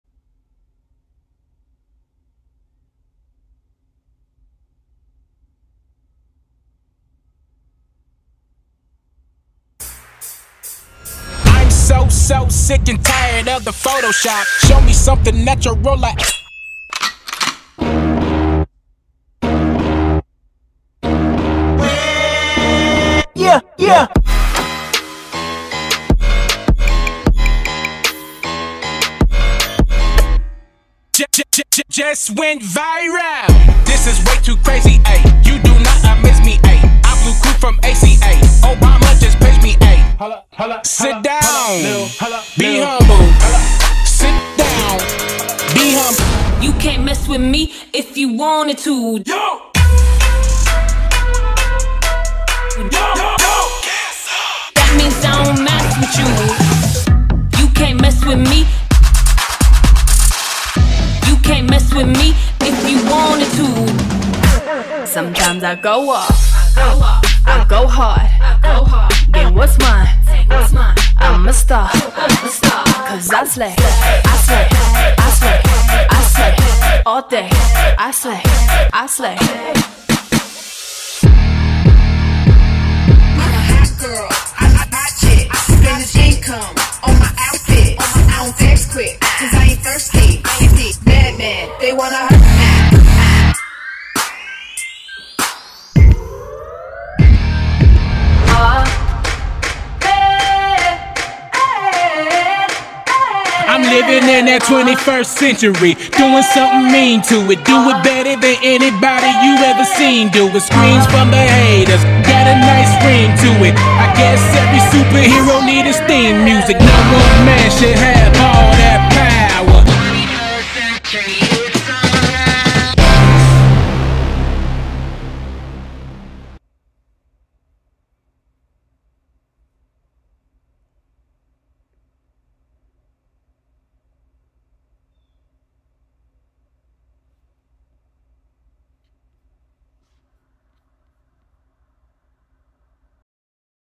Winter Park High School - Hip Hop Nationals 2022
The Roar Dance Team
4th Place Medium Varsity at UDA Nationals
Choreography: Julie Johnson & Kristin Johnson